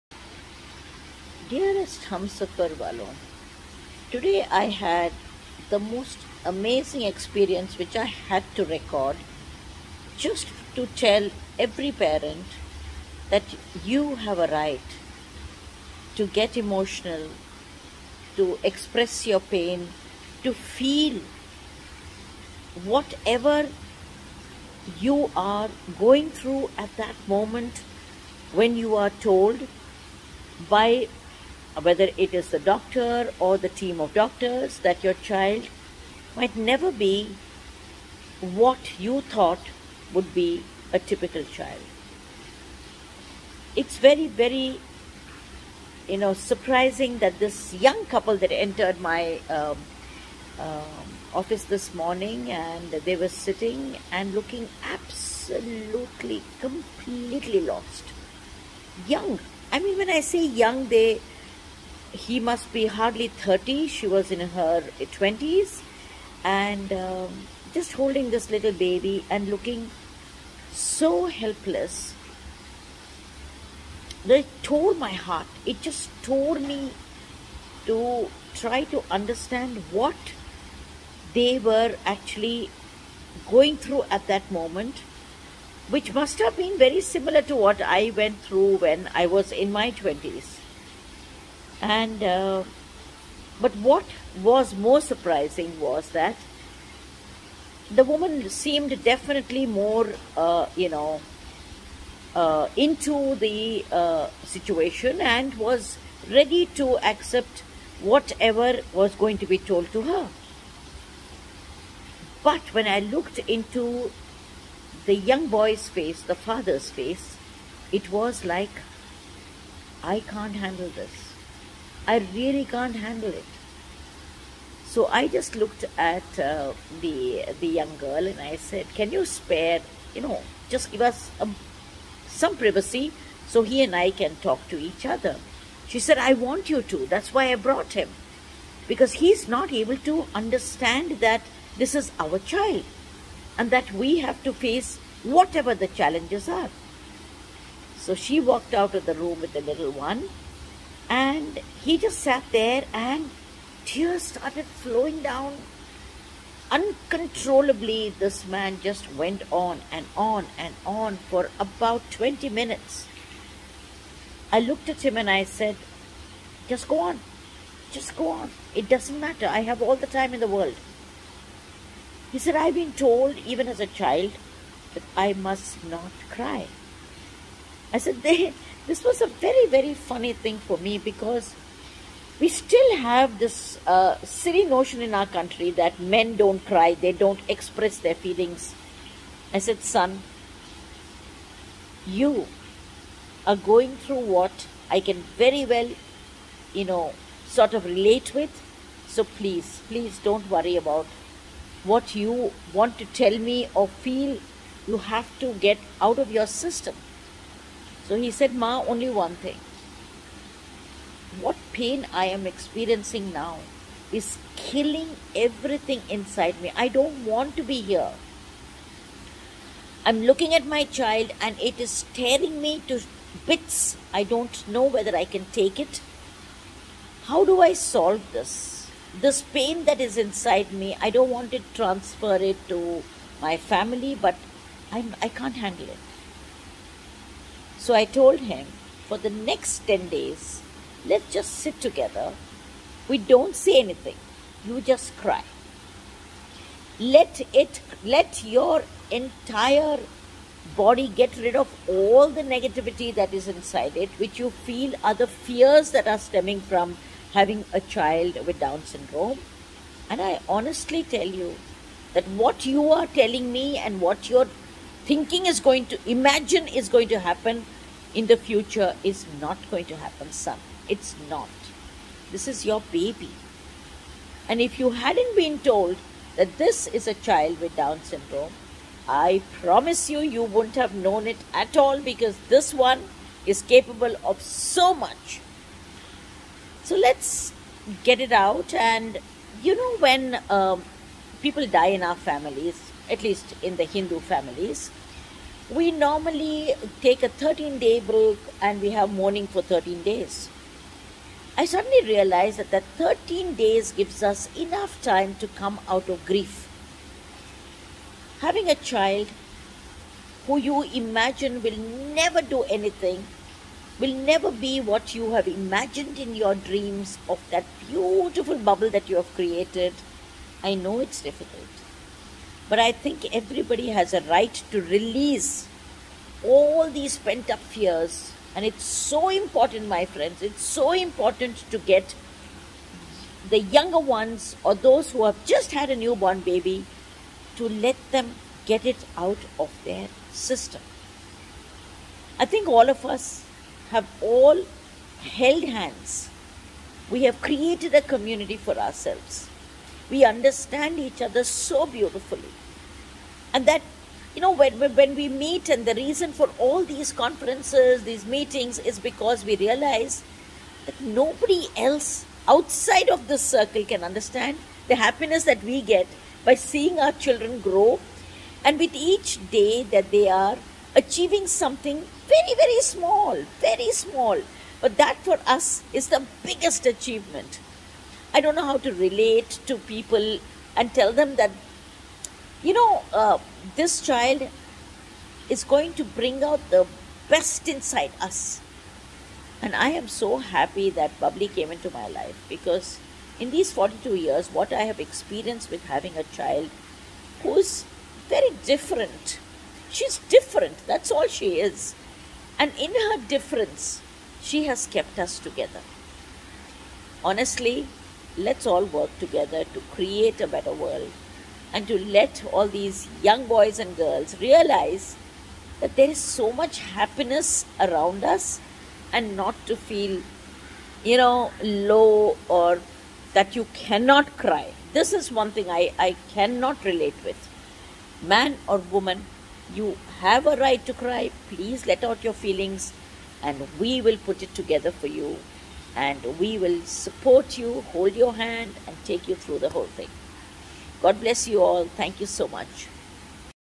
Why Shouldn't Men Cry?
Rekha maa talks about why society doesn't let men cry. This is based on her experience with a new parent. And this experience is something that all of us can relate with.